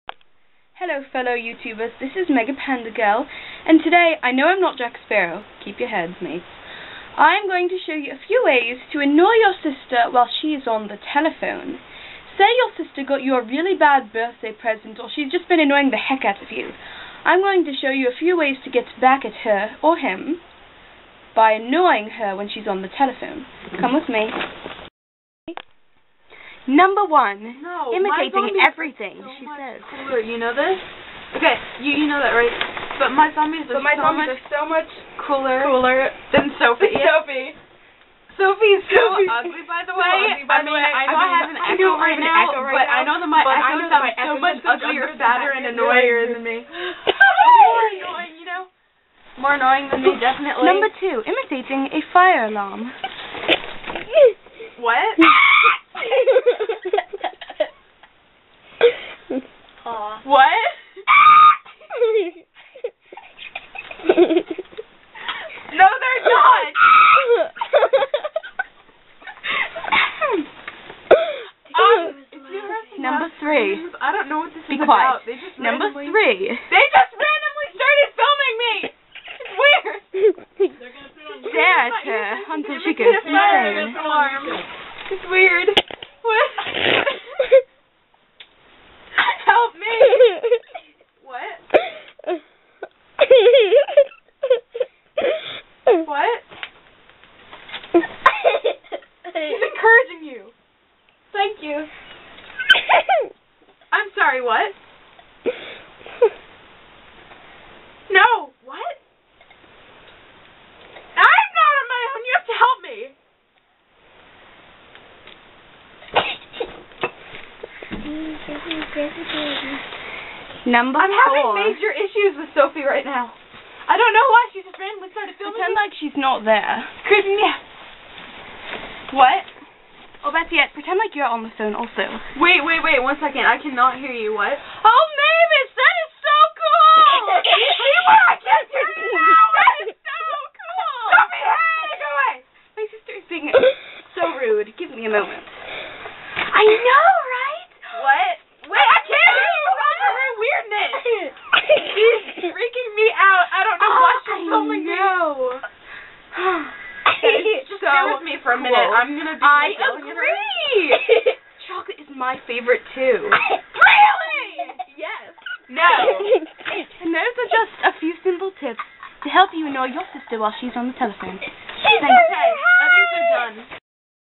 How to Annoy Your Sibling While They're on the Telephone!
This is a step-by-step video guide on how to annoy your sibling (or friend, brother or acquaintance) while they are on the telephone. Hardly any of this was scripted or planned ahead, but my sibling was surprisingly cooperative as I sabotaged this call. Filmed by my little bro.